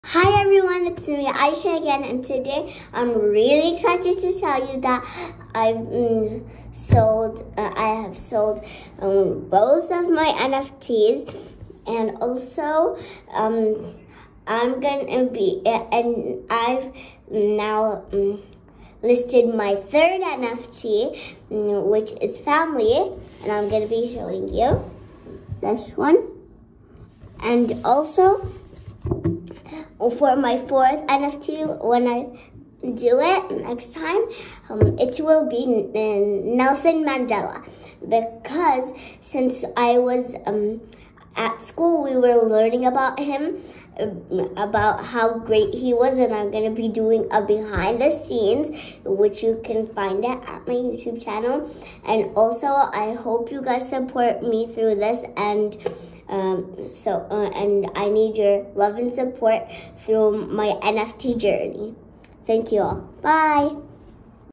I have sold my first 2 NFTs
Aisha : 11-Year-Old Artist Based at United Arab Emirates
 started drawing when she was 7
 she does Traditional and Digital art
sawing many kids joining nft world wanted do her own nfts